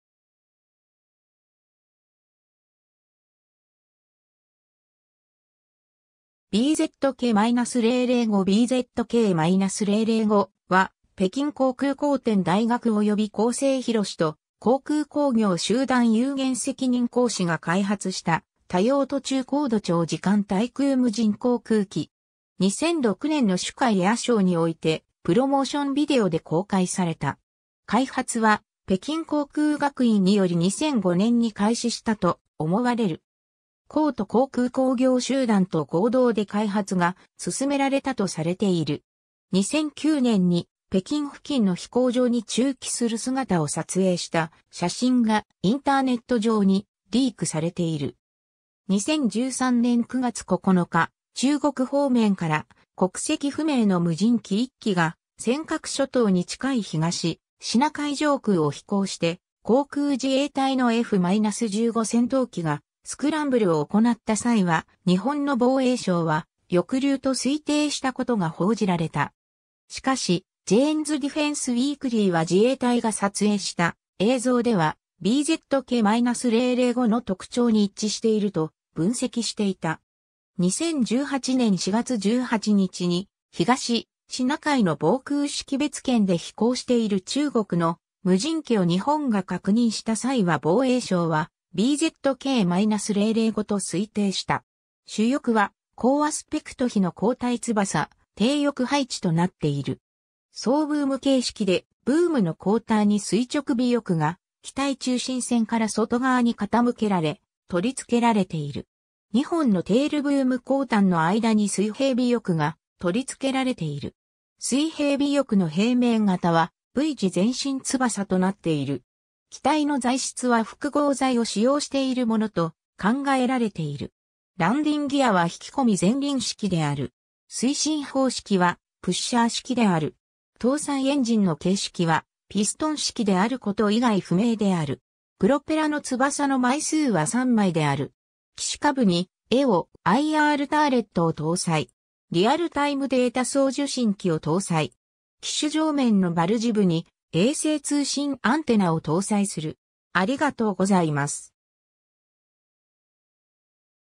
BZK-005 (航空機)
BZK-005 BZK-005は、北京航空航天大学（英語：Beihang University）および江西洪都航空工業集団有限責任公司（英語：Jiangxi Hongdu Aviation Industry Group Corporation Limited、HAIC）が開発した多用途中高度長時間滞空無人航空機(Multirole MALE UAV)。
 2006年の珠海エアショーにおいてプロモーションビデオで公開された。
開発は北京航空学院（BUAA、現北京航空航天大学）により2005年に開始したと思われる。
洪都航空工業集団と合同で開発が進められたとされている。
2009年に北京付近の飛行場に駐機する姿を撮影した写真がインターネット上にリークされている。
 2013年9月9日、中国方面から国籍不明の無人機1機が尖閣諸島に近い東シナ海上空を飛行して航空自衛隊のF-15戦闘機がスクランブルを行った際は日本の防衛省は翼竜と推定したことが報じられた。
しかし、ジェーンズ・ディフェンス・ウィークリーは自衛隊が撮影した映像ではBZK-005の特徴に一致していると分析していた。
2018年4月18日に東シナ海の防空識別圏で飛行している中国の無人機を日本が確認した際は防衛省はBZK-005と推定した。
 主翼は高アスペクト比の後退翼、低翼配置となっている。
双ブーム形式でブームの後端に垂直尾翼が機体中心線から外側に傾けられ取り付けられている。
2本のテールブーム後端の間に水平尾翼が取り付けられている。
水平尾翼の平面形はV字前進翼となっている。
機体の材質は複合材を使用しているものと考えられている。
ランディンギアは引込み前輪式である。
推進方式はプッシャー式である。
搭載エンジンの型式は、ピストン式であること以外不明である。
プロペラの翼の枚数は3枚である。
 機首下部にEO/IRターレットを搭載。
リアルタイムデータ送受信機を搭載。
 機首上面のバルジ部に衛星通信アンテナを搭載する。